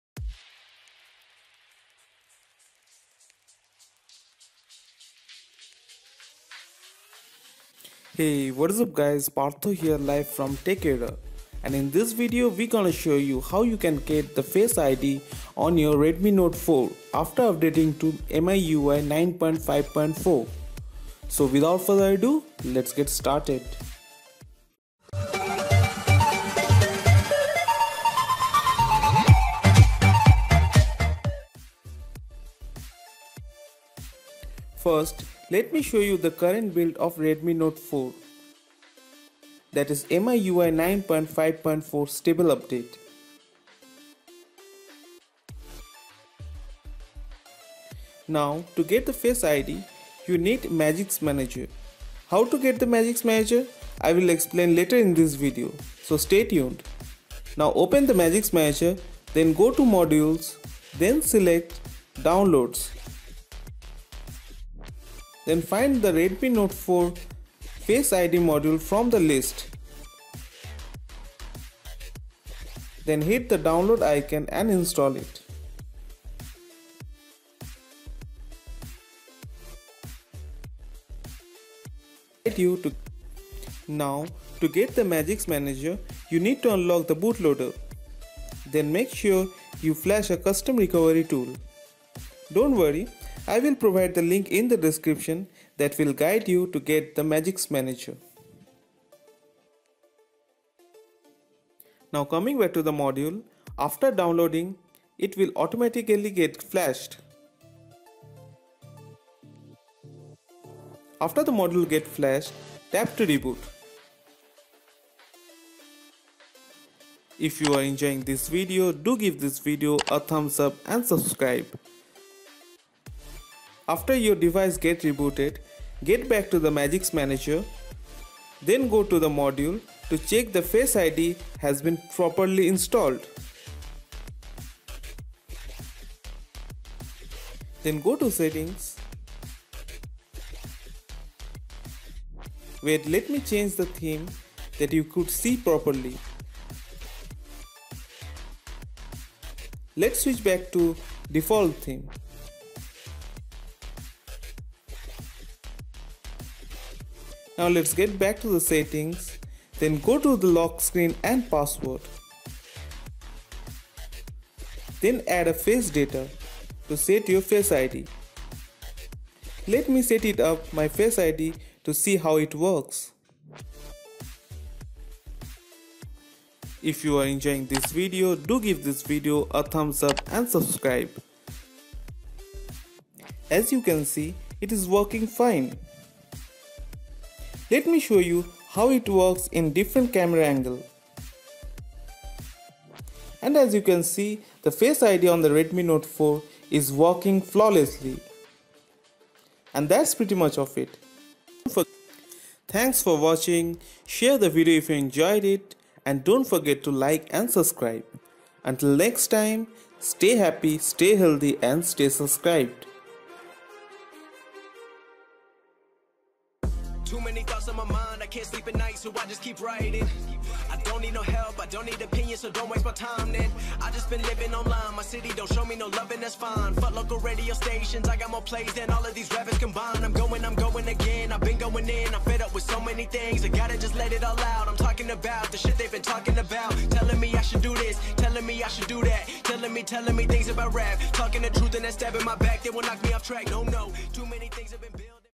Get Face Unlock Feature On Redmi Note 4 After MIUI 9.5 | Stable Update | Same As Redmi Note 5 Pro !
Get Face Id / Face Unlock Hidden Feature On Redmi Note 4x After Updating To MIUI Global 9.5.4.0 | Stable Update. Same As Redmi Note 5 Pro Or One Plus 5T

HOW TO GET MAGISK MANAGER OR How To Switch From SuperSU To Magisk

Credits:
~Bazanji